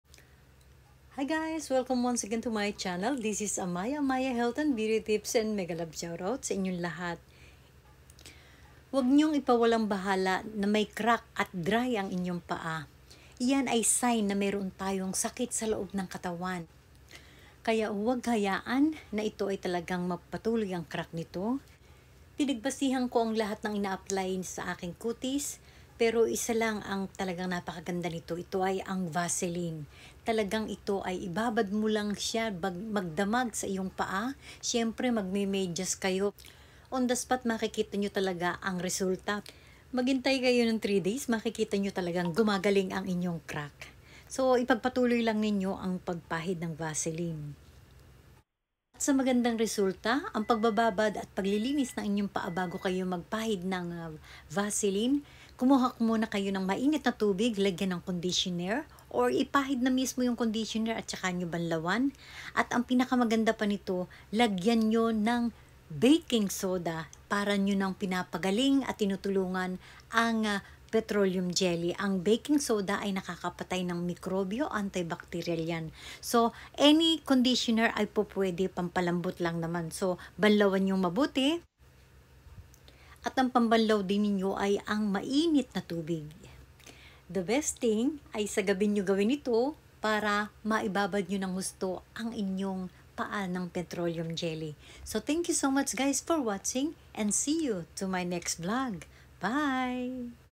Paano Gamutin ang Crack Heels or Tibak sa Paa
ano ang gamot sa tibak sa paa
paano gamutin ang crack sa paa